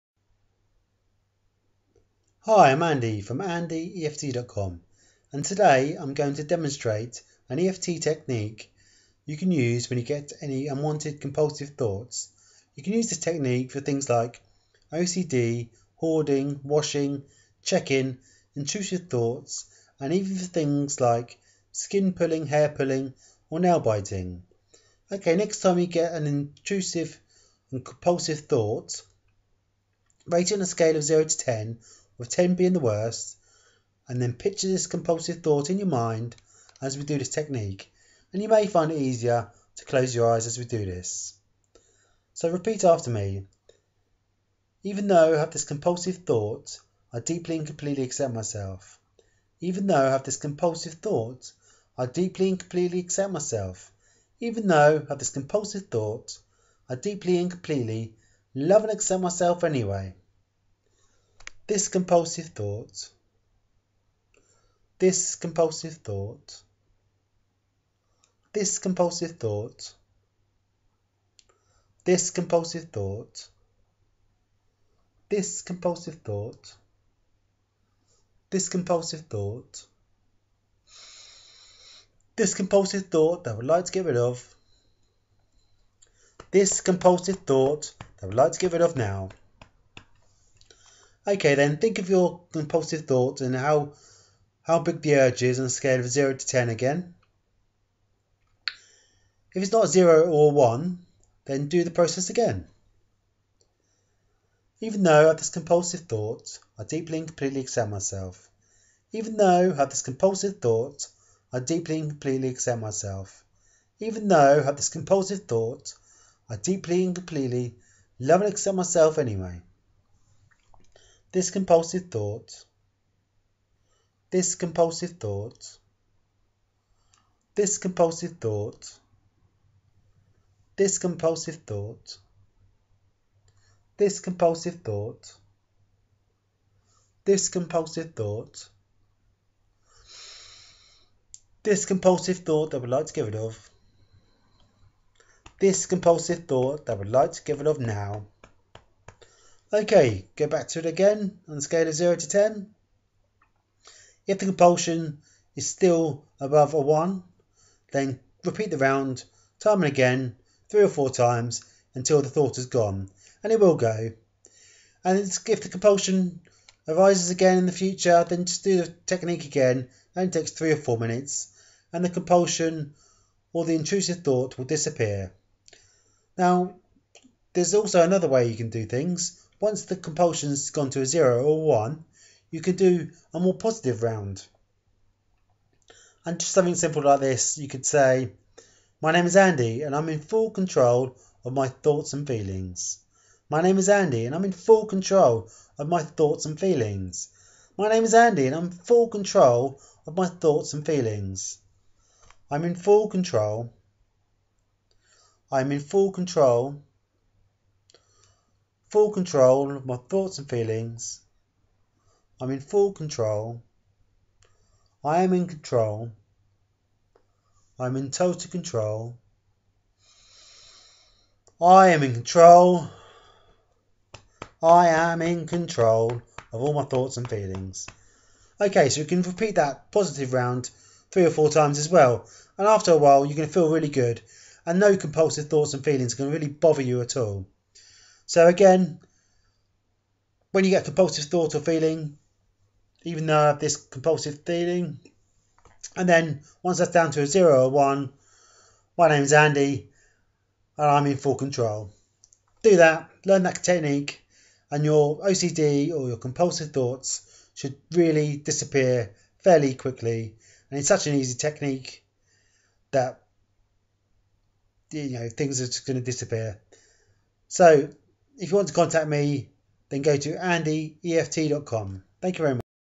STOP THOSE COMPULSIVE THOUGHTS NOW! - with this easy and quick technique
EFT (Emotional Freedom Technique) demonstration for compulsive/intrusive thoughts, OCD, checking, washing, hoarding, Trichotillomania, Dermatillomania, nail-biting etc.

Learn off-by-heart and use everytime one of these thoughts/feelings/urges appears.